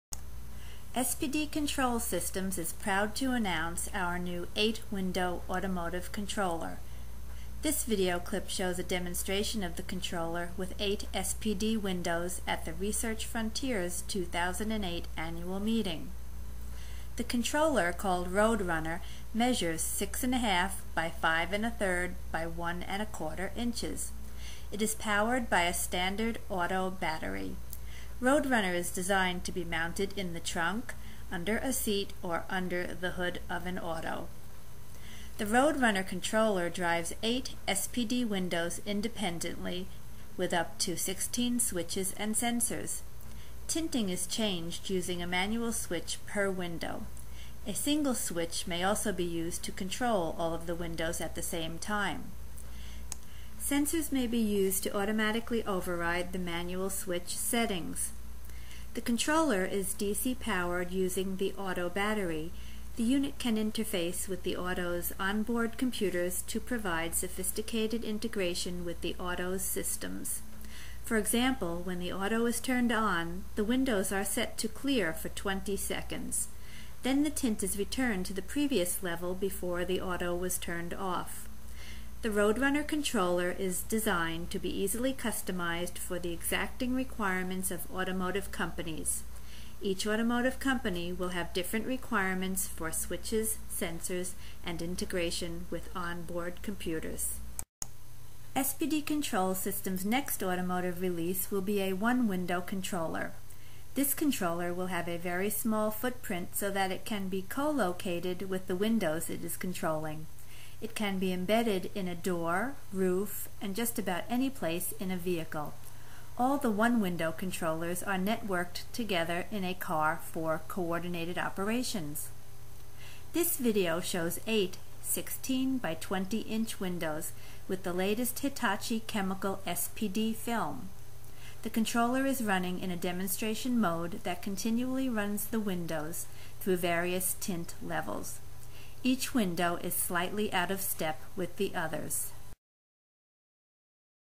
SPD Window Controller Demonstration
The Automotive Demonstration video was recorded at the 2008 Research Frontiers, Inc. (RFI) Annual Meeting.   The film used is from Hitachi with light transmittance from 0.4% to 52%.  The film is remarkable for it range of transmittance and quality.